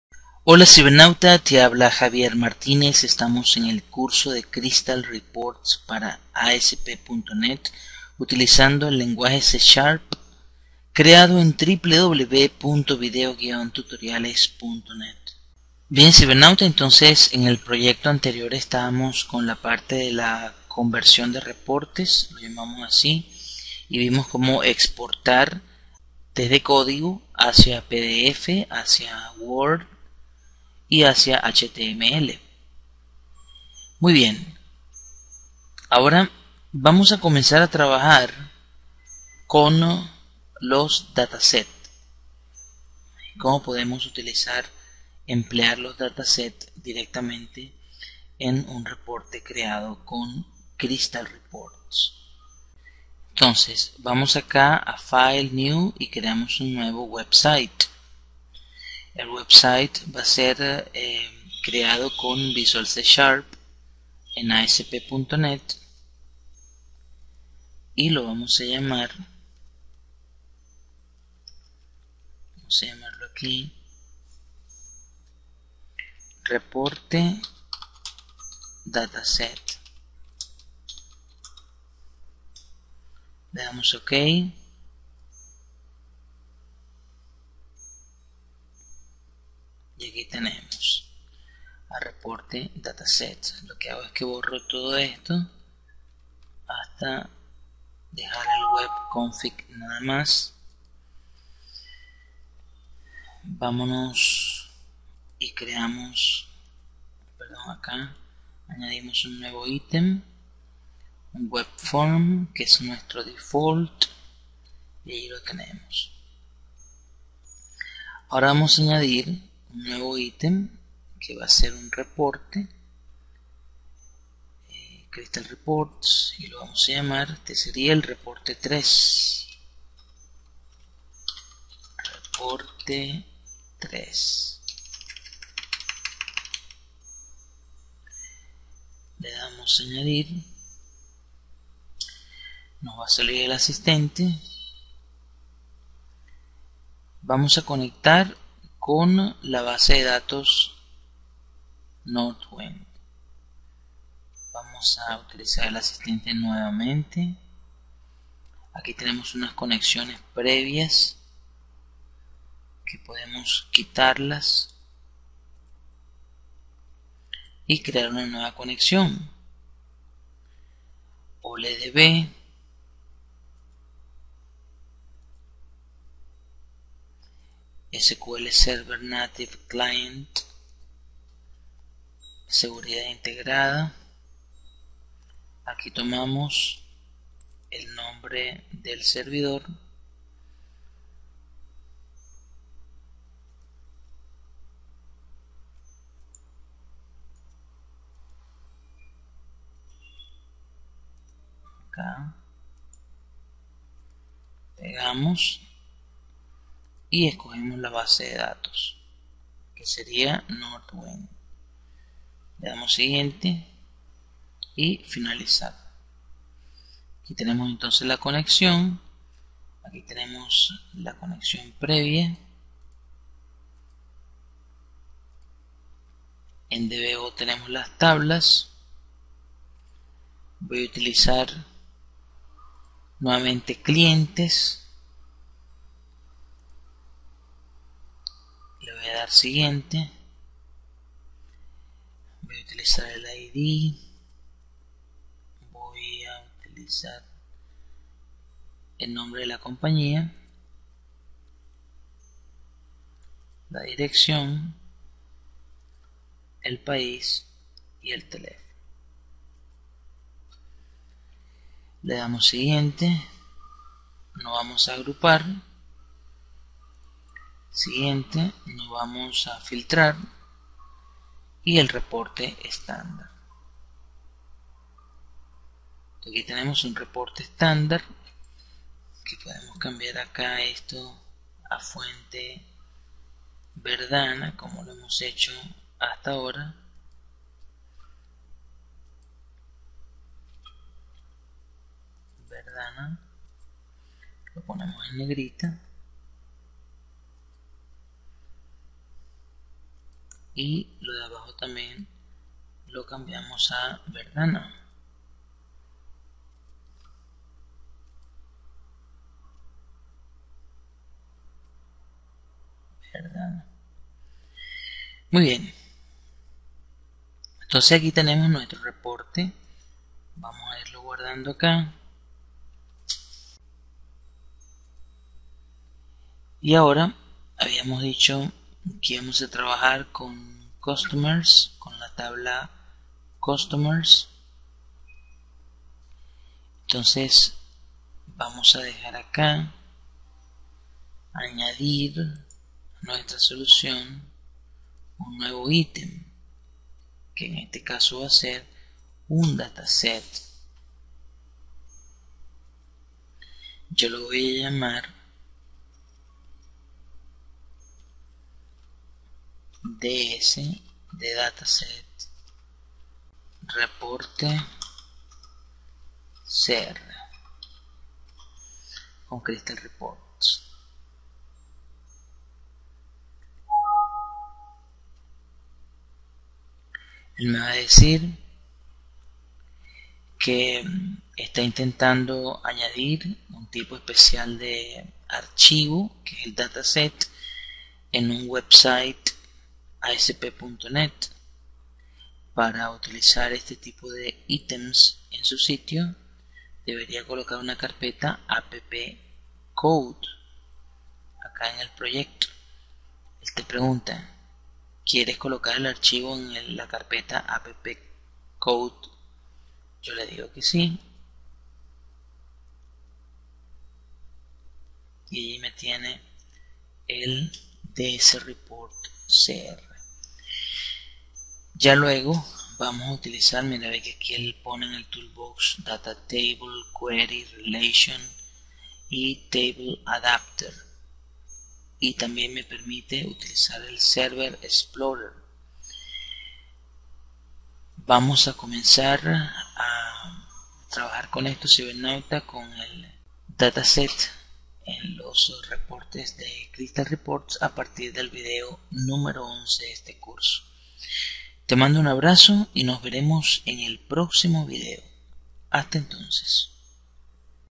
(DEMO) CURSO DE CRYSTAL REPORTS CON ASP .NET Y C# - VIDEO 10
Crear un nuevo website dentro de Visual Studio. Hacemos los ajustes. Añadimos el reporte3.rpt con Crystal Reports y su asistente. Añadimos un DataSet que vamos a enlazar con la tabla Customers en el Video 11.

ESTE CURSO NO ES GRATUITO. PARA OBTENERLO DEBES SER SUSCRIPTOR VOLUNTARIO Y COMUNICARTE CON MI PERSONA PARA QUE TE APROVECHES DE LA OFERTA DEL PACK .NET (DESKTOP o WEB).

Dedicado a mi querida hermana y mejor amiga.

Por tu éxito:

Javier J. Martínez F.